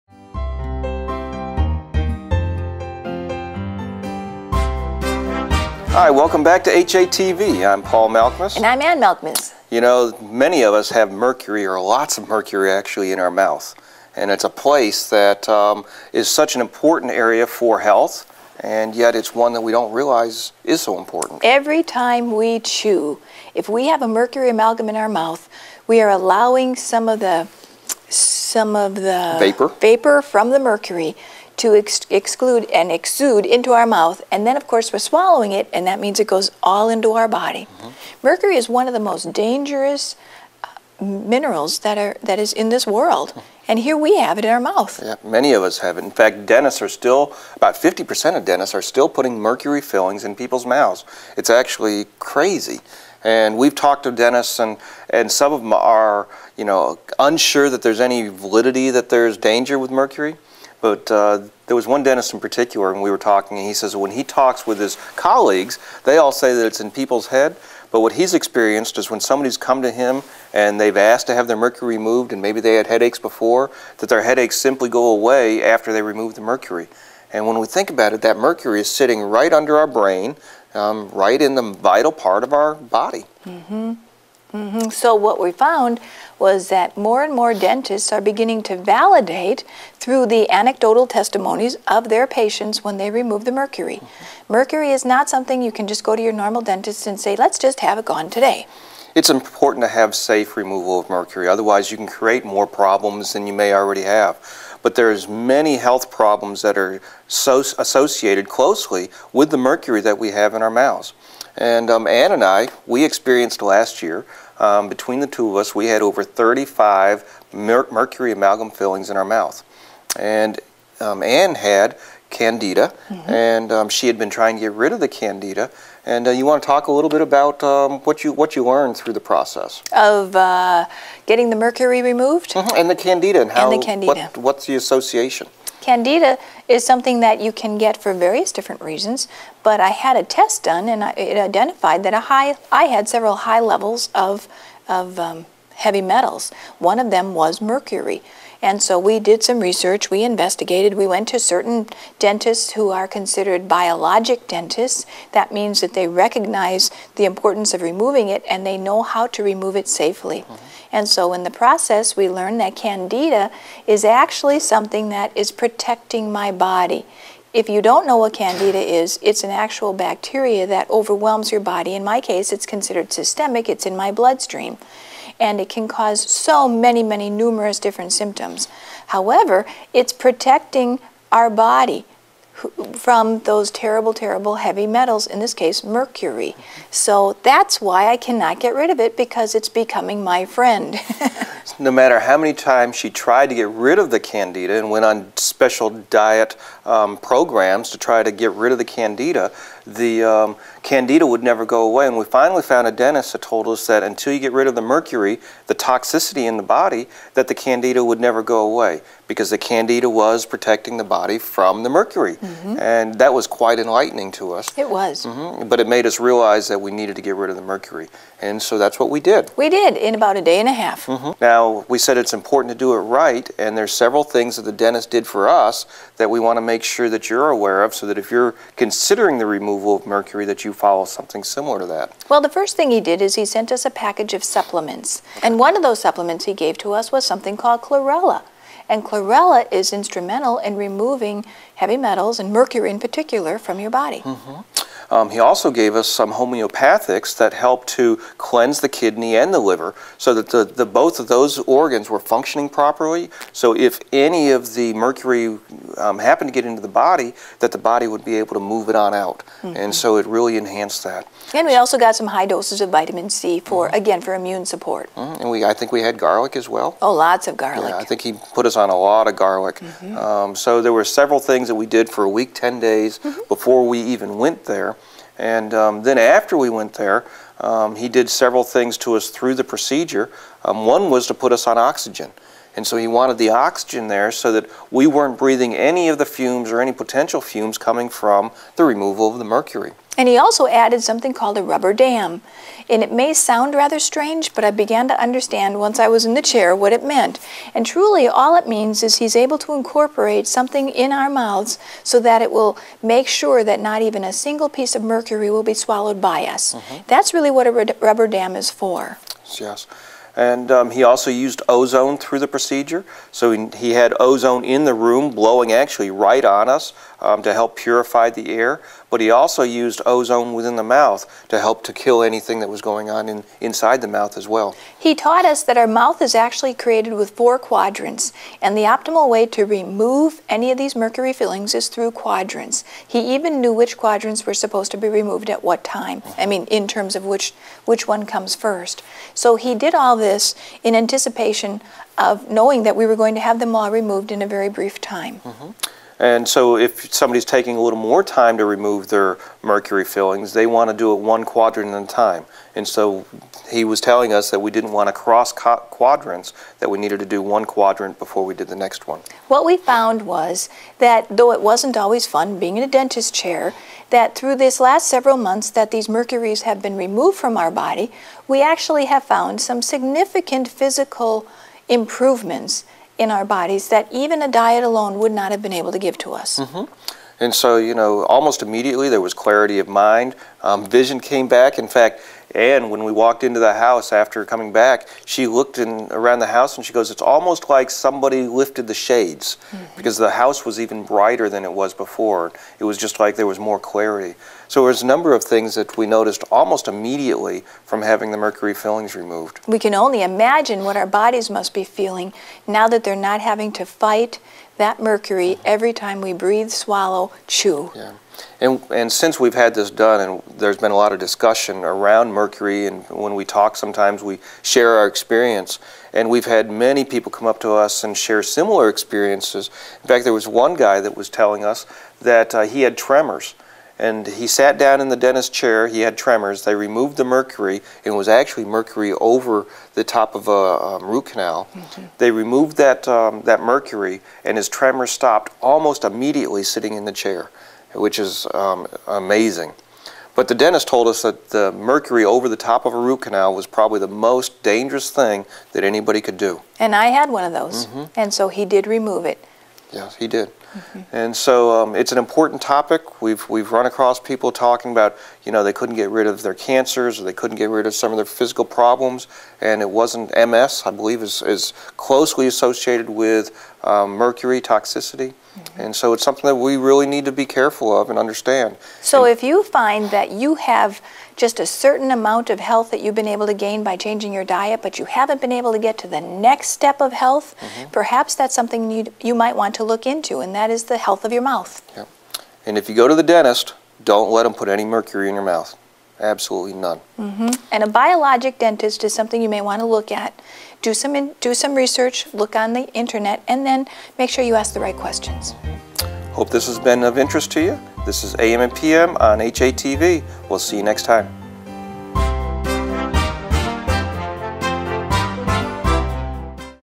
Mercury Dental Fillings
What we put into our bodies has always been important, whether it is food related or medically. For many years, dentists have regularly inserted mercury fillings into cavities and other dental procedures. However, as more research is done. we are finding out just how bad that mercury can actually be for your health. Join Paul and Ann as they discuss the dangers of mercury fillings, and talk about their own expriences dealing with them.